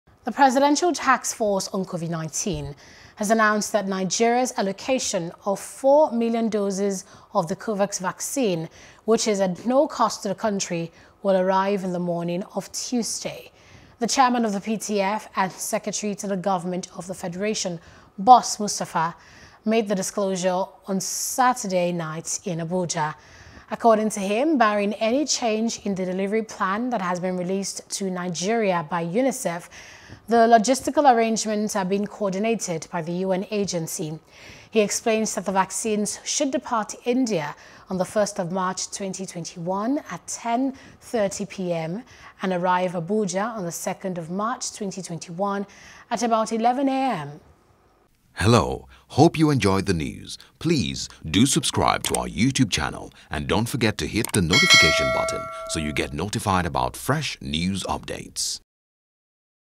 Covid -19 Vaccine Arrives Tomorrow – FG | NIGERIA
Presidential Task Force on COVID-19, has announced that Nigeria’s allocation of four million doses of the COVAX vaccine which is at no cost to the country will arrive in the morning of Tuesday.